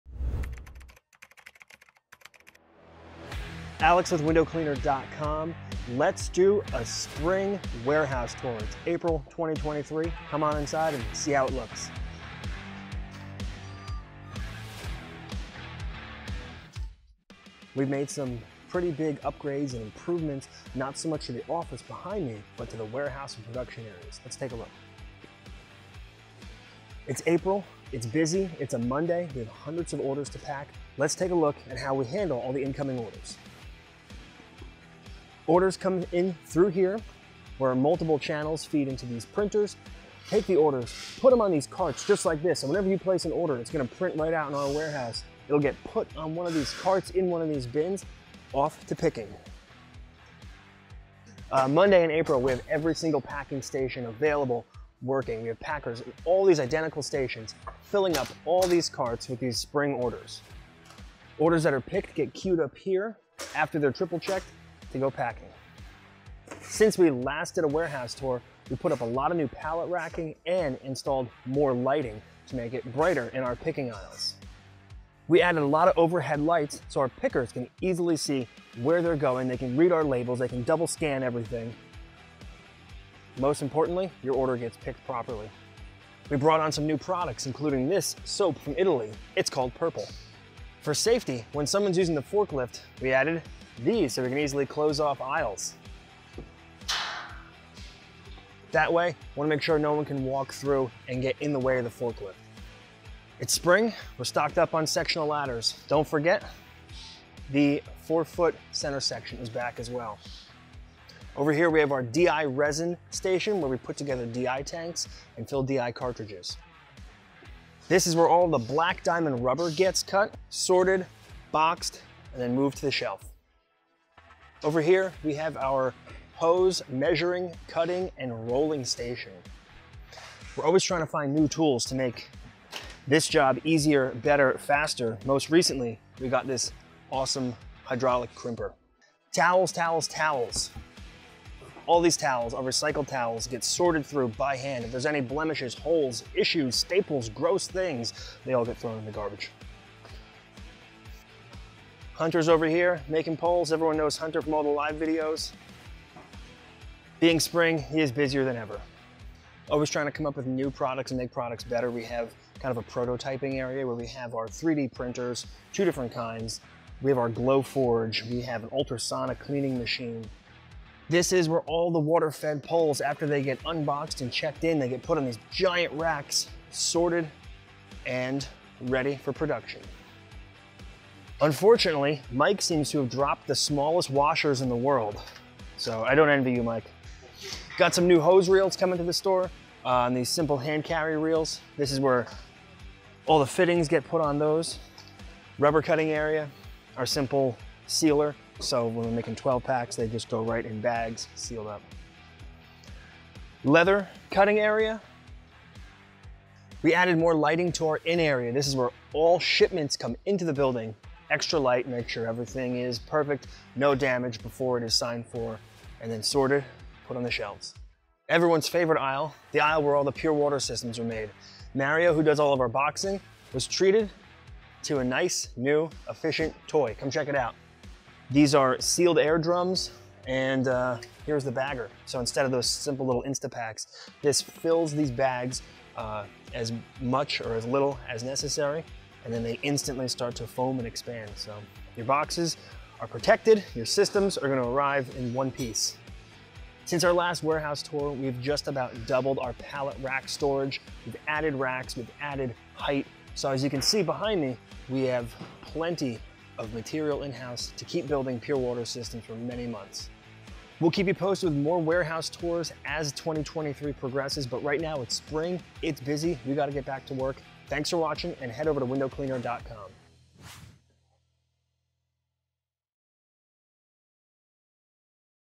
WCR Warehouse Tour | Spring 2023
Here at Window Cleaning Resource, we are always shaking things up to see how we can become better, faster, and more efficient, which means adding and subtracting elements of our warehouse!